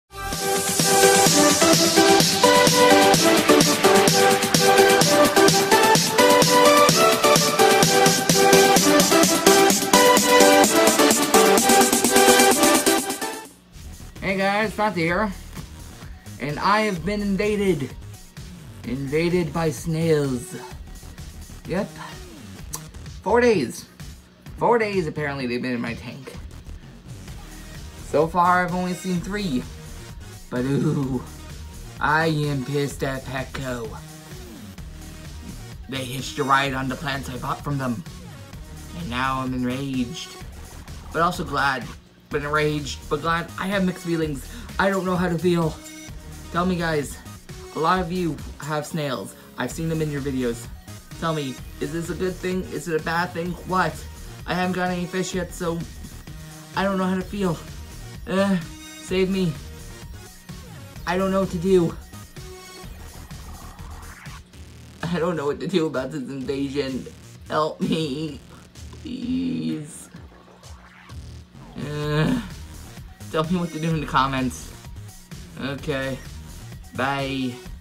25 Gallon Update | Snail Invasion
Need advice on this should I keep them or get rid of them, and species of snails are they...the help is appropriated

Invasion log:
January 9th, 2019 - Count of snails is 5
January 10th, 2019 - Count of snails has dropped to 2-3 individuals; unsure if the others have died or are just out of sight